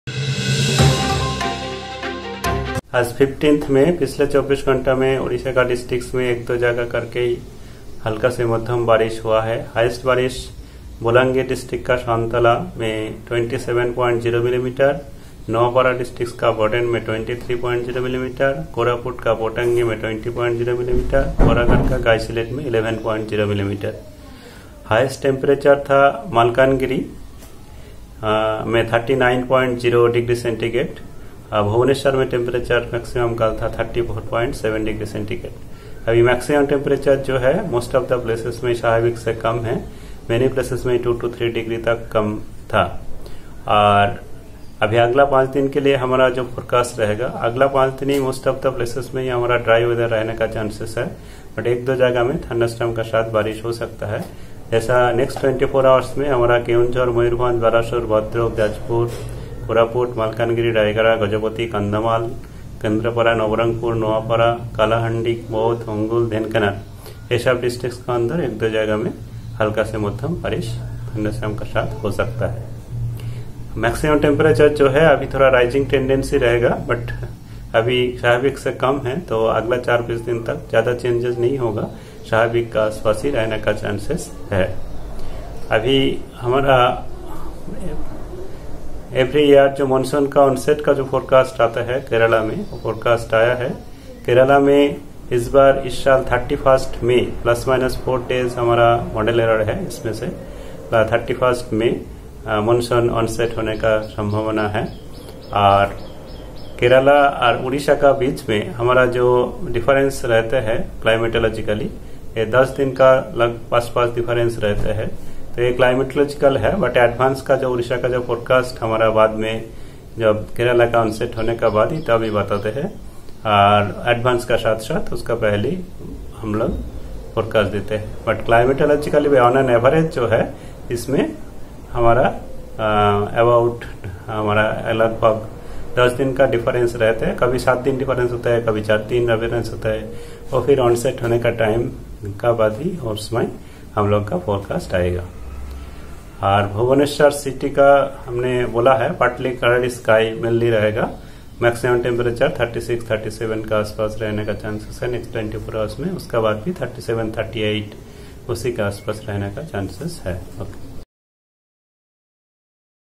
Weather briefing for next five days by Head & Scientist-E: Dr. H.R. Biswas
Weather briefing by Head & Scientist-E Dr. H.R. Biswas for next five days, based on 0830 Hrs IST observation of 15th May 2021.